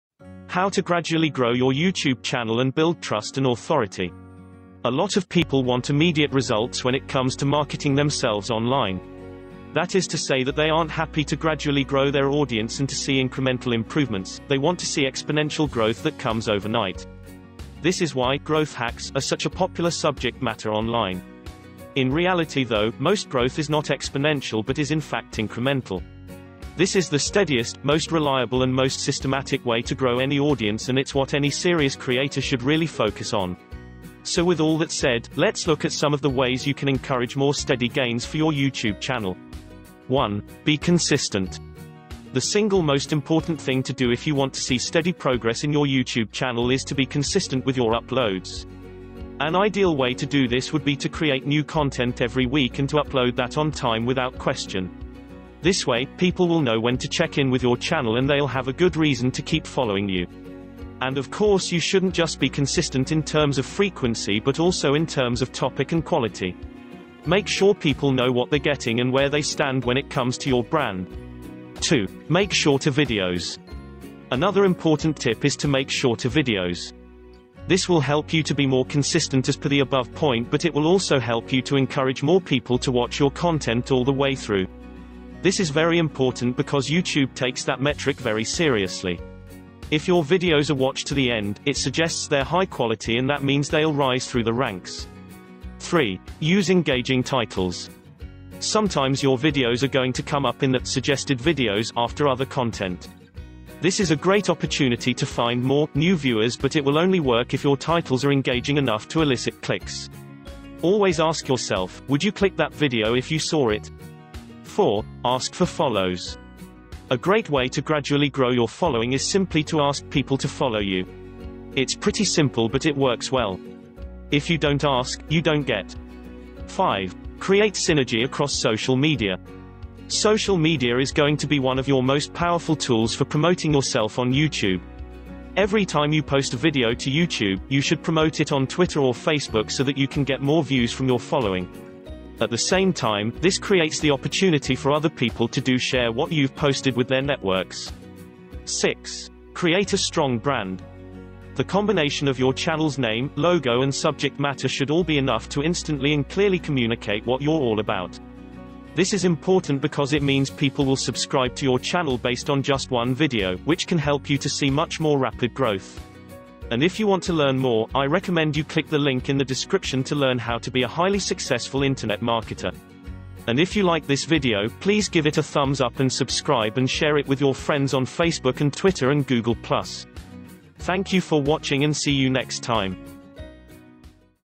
How to Gradually Grow Your YouTube Channel and Build Trust and Authority.
A lot of people want immediate results when it comes to marketing themselves online. That is to say that they aren’t happy to gradually grow their audience and to see incremental improvements – they want to see exponential growth that comes overnight. This is why ‘growth hacks’ are such a popular subject matter online!

In reality though, most growth is not exponential but is in fact incremental. This is the steadiest, most reliable and most systematic way to grow any audience and it’s what any serious creator should really focus on.

So with all that said, let’s look at some of the ways you can encourage more steady gains for your YouTube channel.

1. Be Consistent.

The single most important thing to do if you want to see steady progress in your YouTube channel is to be consistent with your uploads. An ideal way to do this would be to create new content every week and to upload that on time without question. This way, people will know when to check in with your channel and they’ll have a good reason to keep following you!

And of course you shouldn’t just be consistent in terms of frequency but also in terms of topic and quality. Make sure people know what they’re getting and where they stand when it comes to your brand!

2. Make Shorter Videos.

Another important tip is to make shorter videos. This will help you to be more consistent as per the above point but it will also help you to encourage more people to watch your content all the way through. This is very important because YouTube takes that metric very seriously. If your videos are watched to the end, it suggests they’re high quality and that means they’ll rise through the ranks!

3. Use Engaging Titles.

Sometimes your videos are going to come up in the ‘Suggested Videos’ after other content. This is a great opportunity to find more, new viewers but it will only work if your titles are engaging enough to elicit clicks. Always ask yourself: would you click that video if you saw it?

4. Ask for Follows.

A great way to gradually grow your following is simply to ask people to follow you! It’s pretty simple but it works well. If you don’t ask, you don’t get!

5. Create Synergy Across Social Media.

Social media is going to be one of your most powerful tools for promoting yourself on YouTube. Every time you post a video to YouTube, you should promote it on Twitter or Facebook so that you can get more views from your following. At the same time, this creates the opportunity for other people to do share what you’ve posted with their networks.

6. Create a Strong Brand.

The combination of your channels name, logo and subject matter should all be enough to instantly and clearly communicate what you’re all about.